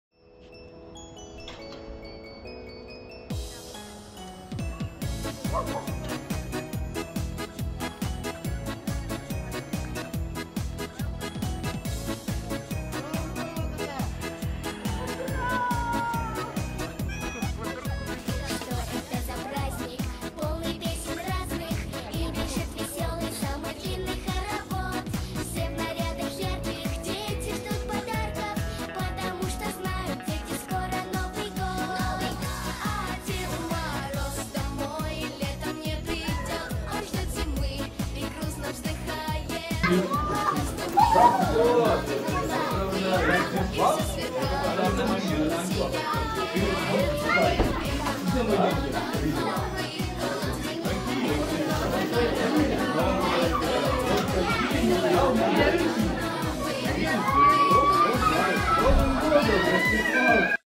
Крымские спасатели в костюмах Дедов Морозов спустились с крыши к малышам в симферопольской больнице
Необычное поздравление для малышей, которые сейчас получают лечение в Республиканской детской клинической больнице, организовали крымские спасатели.

Самые актуальные новости Крыма и России на официальном сайте "ИА Крымский Телеграф"